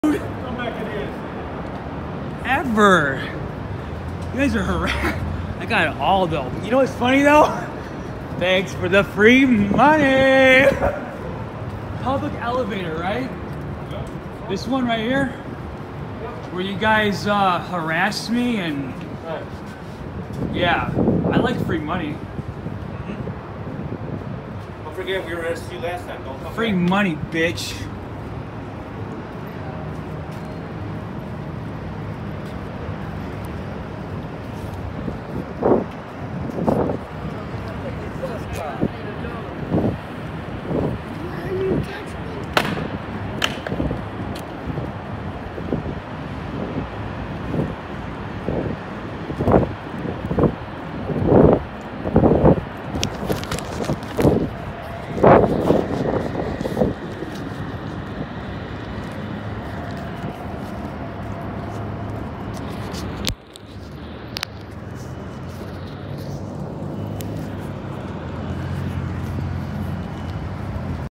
Sovereign citizen The police put hands on me in the elevator and tried breaking my phone
Free money bitch